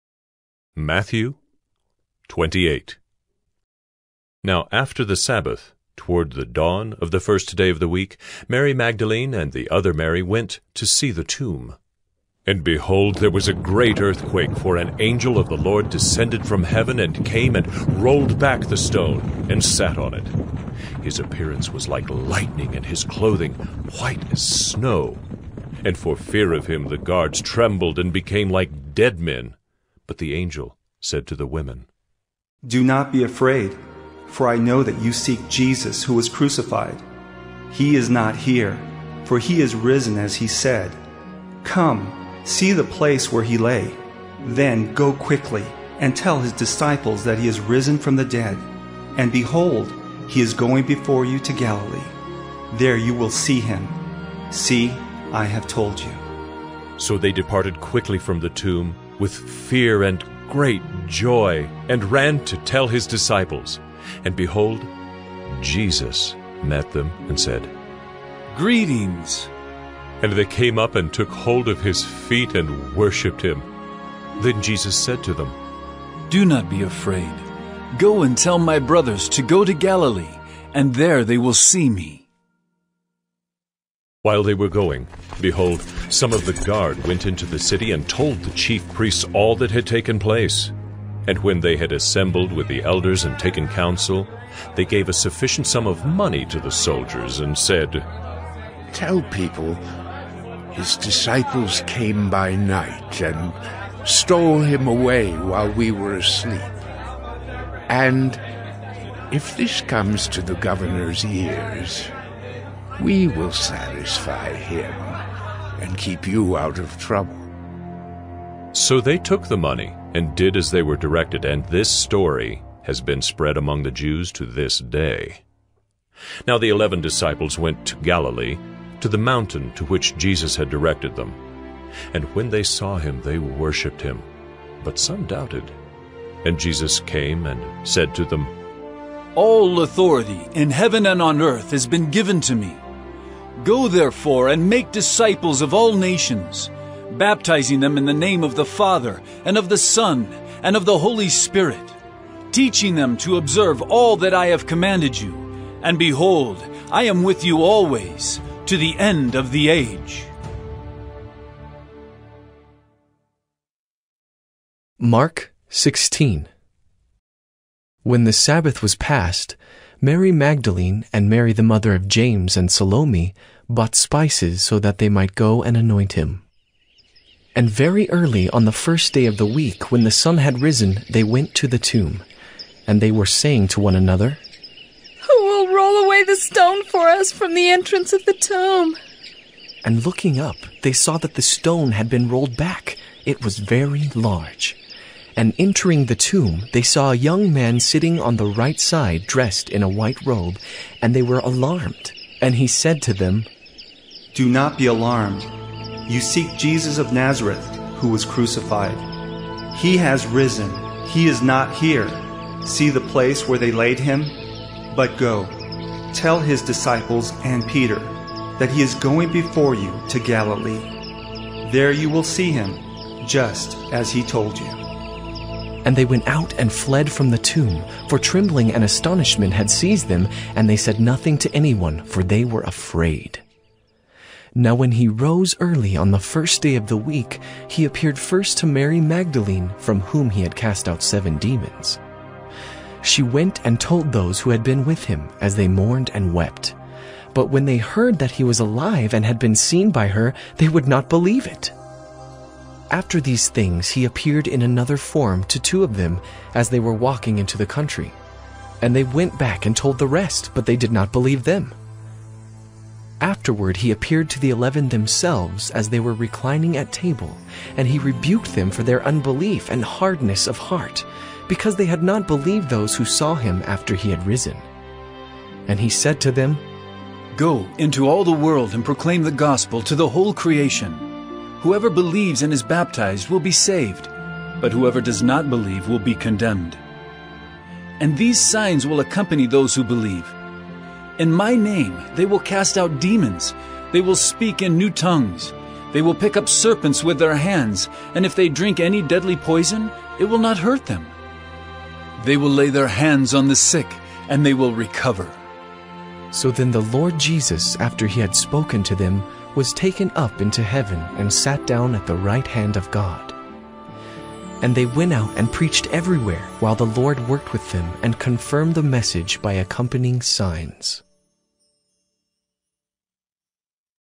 Day 318 - ESV Dramatized Audio - One Year Chronological Daily Bible Reading Plan - Nov 14
Gain better understanding of the order of biblical events and the historic context in which they unfolded.

Chronological plan by Blue Letter Bible is compiled according to recent historical research, taking into account the order in which the recorded events actually occurred.  With the full text divided into 356 portions for daily reading it allows readers the opportunity to read through the Bible in one year. If the schedule provided is followed, the entire Bible will be read in one calendar year.

Chronological Daily Bible Reading plan is by the Blue Letter Bible